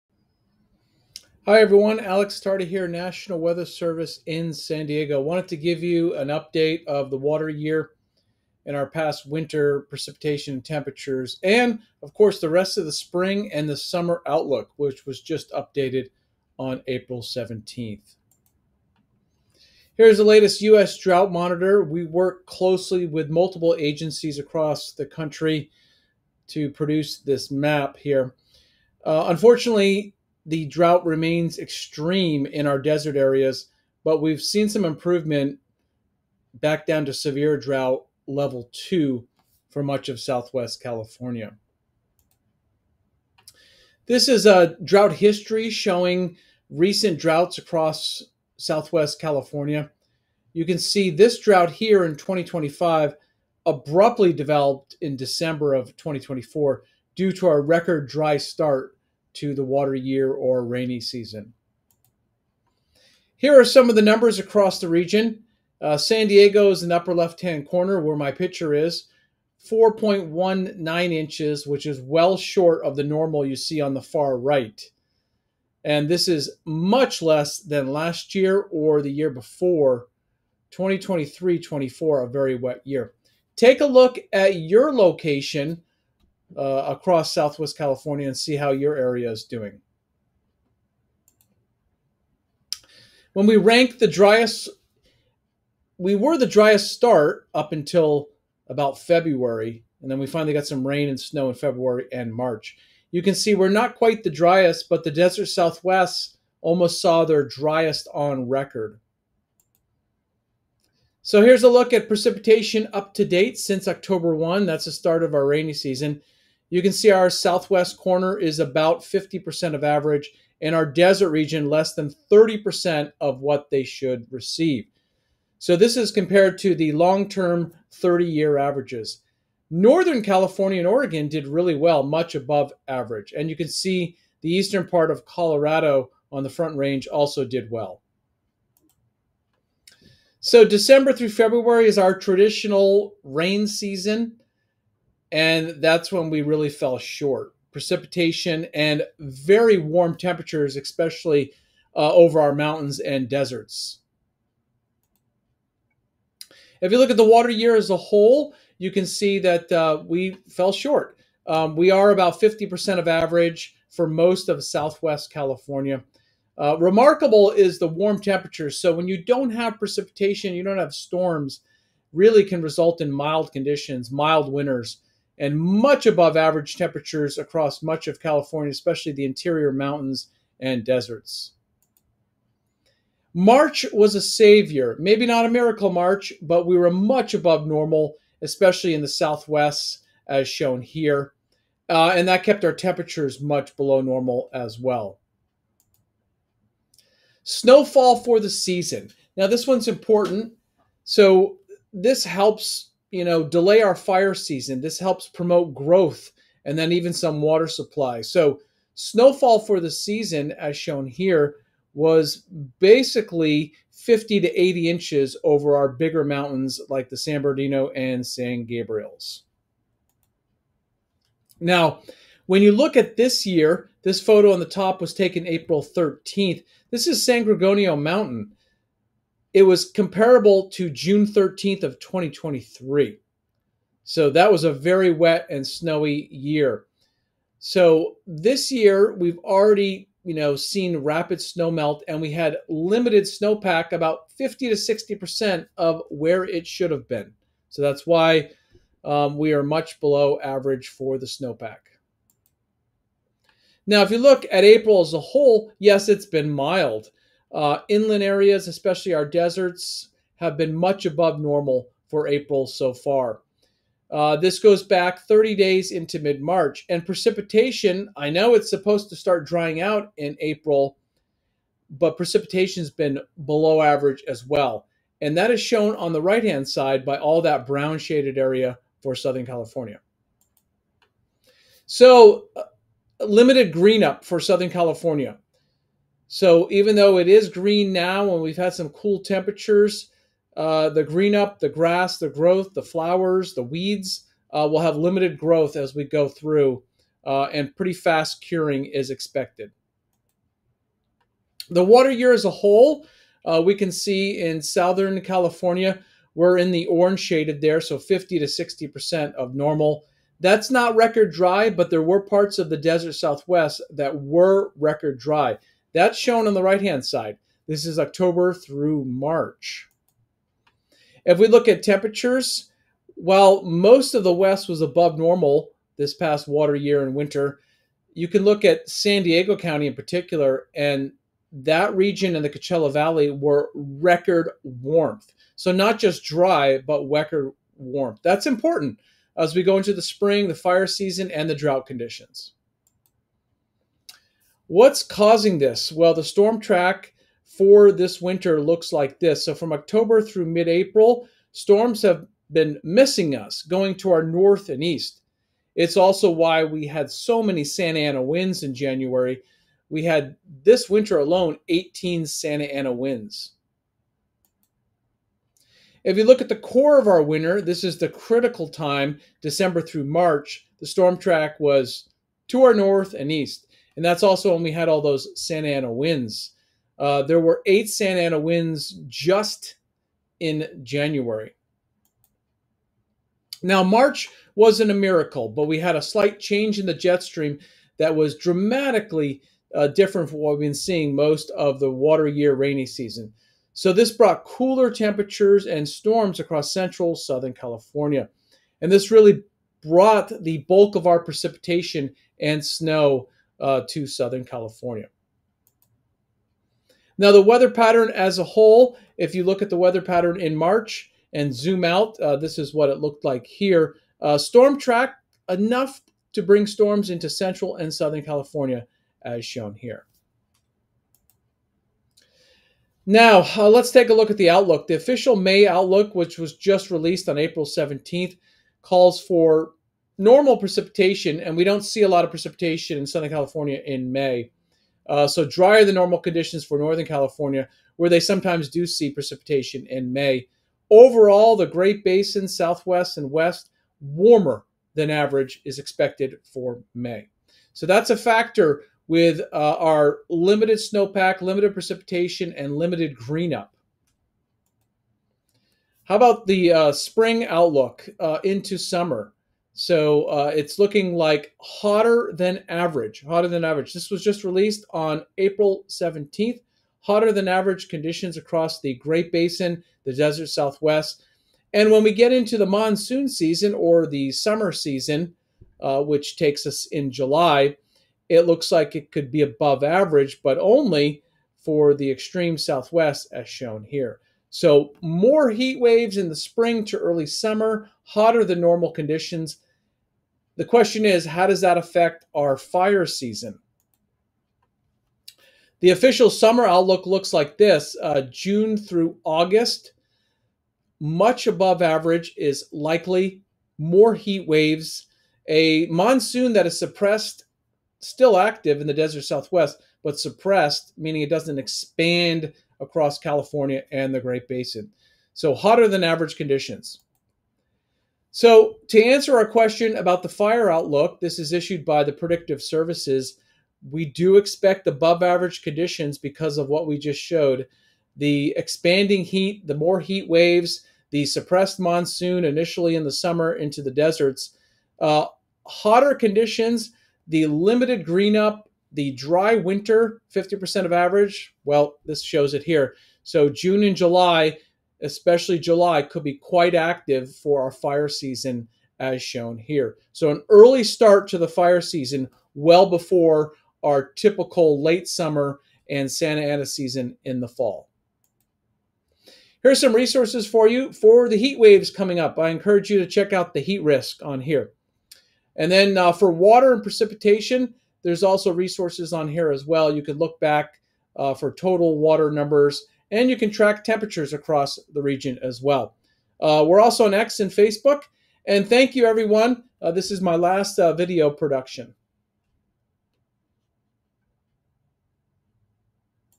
Water Year 2025 and Spring to Summer outlook - NWS San Diego
An updated Spring and Summer outlook for Southern California, along with the latest information for Water Year 2024-25.